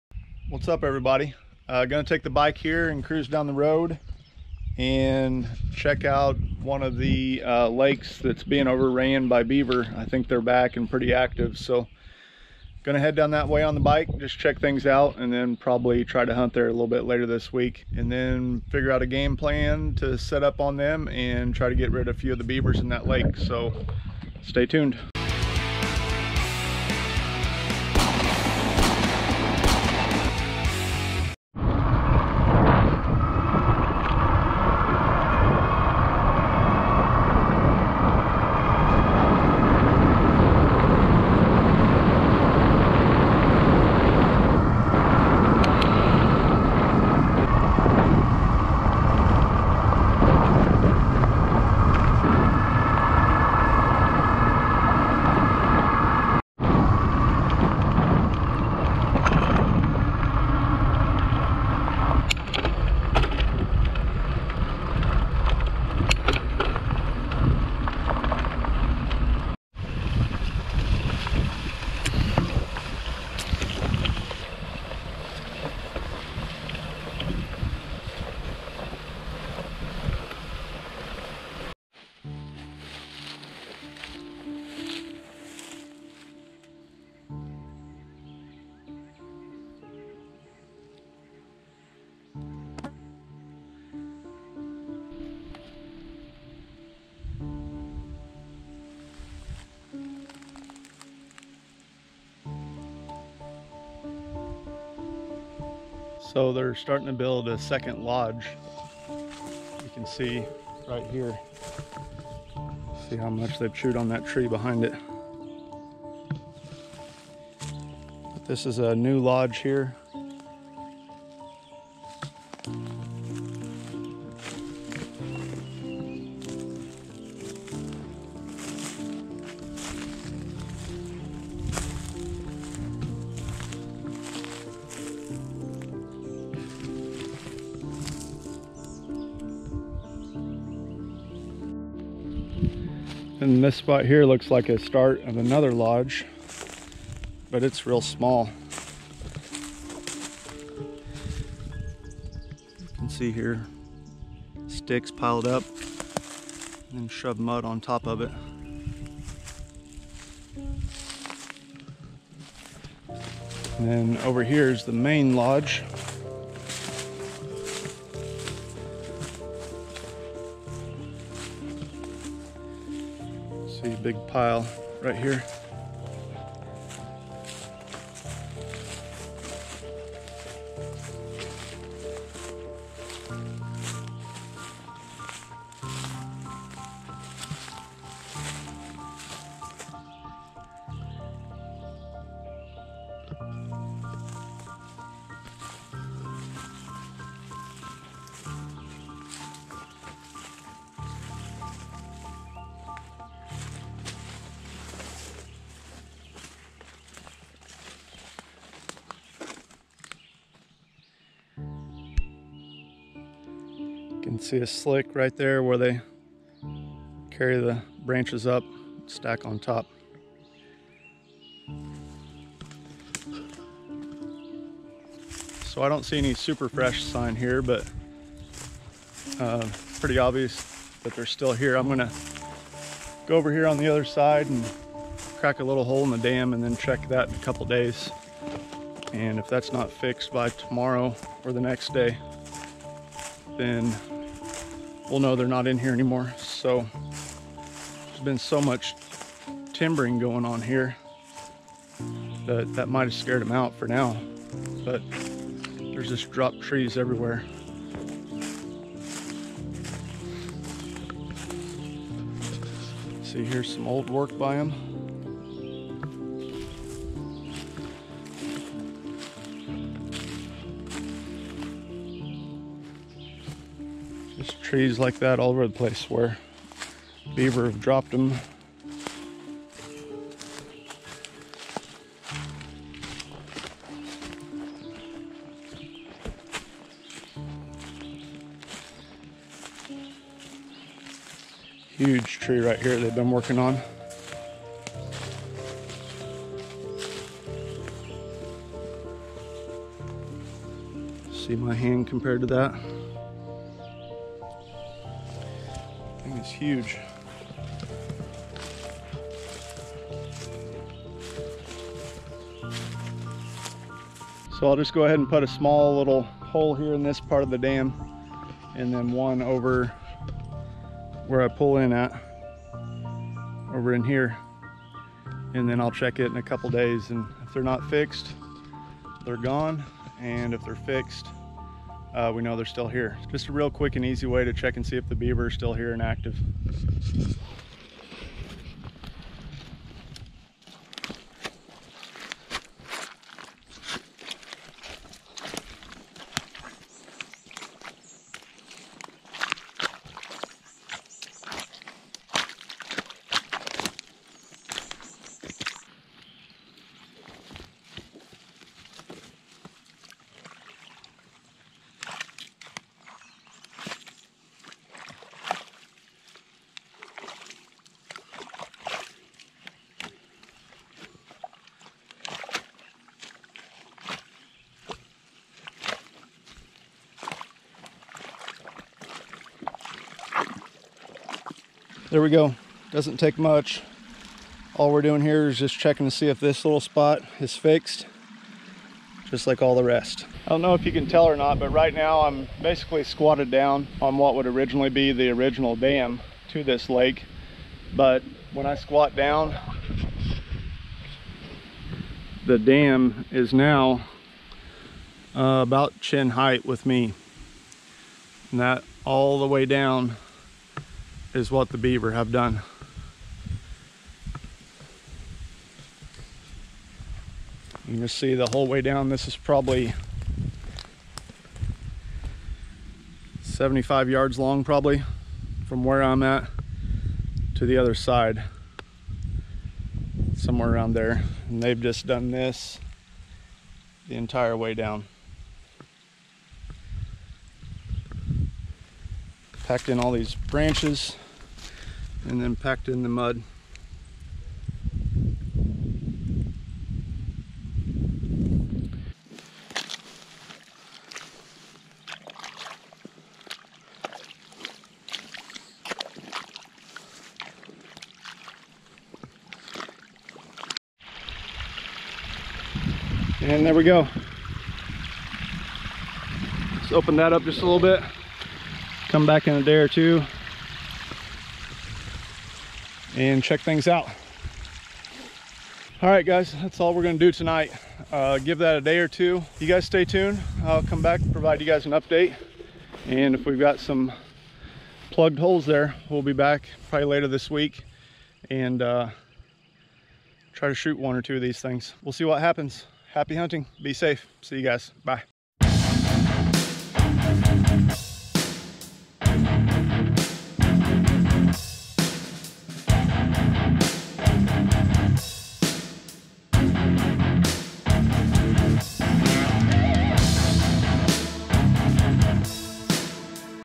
Beaver - Where They At?
Went out to scout a local lake to try and locate some beaver.  Not much fresh activity but I opened up the dam in a few spots to try and draw them in to work on it.  I will be back for an updated video to see if they have fixed the holes I put in the dam.

Main Camera
Camera Lens
Camera Lens 2
Secondary Camera
My GoPro
My Drone
My Vlogging Tripod